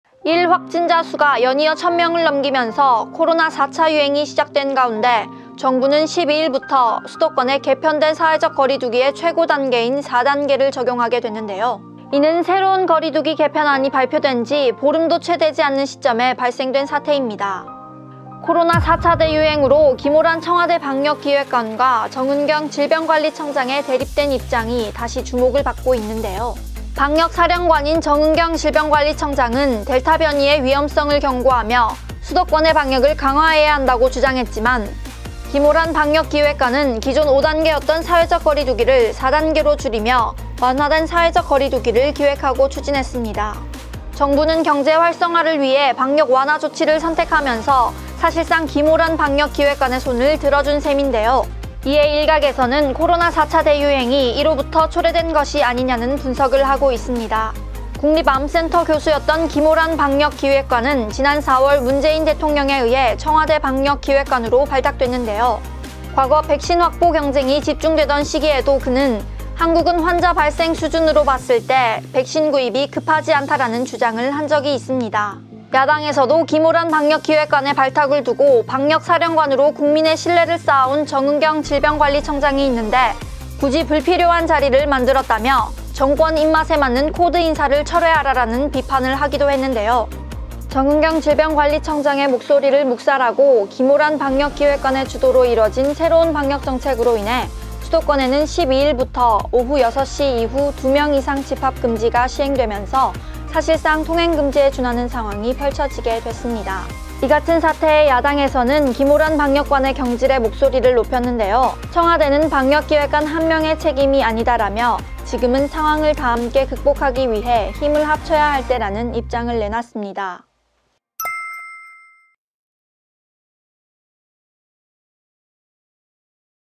4차 대유행은 막을 수 있었다? 방역 완화 주도한 기모란 靑 방역 기획관
4차 대유행은 막을 수 있었다?
방역 완화 주도한 기모란 靑 방역 기획관 VS 방역 사령관 정은경 청장

과거 백신 급하지 않다던 기모란 방역 기획관의 발언에 이어
두 번째 방역 실패 사례인데요.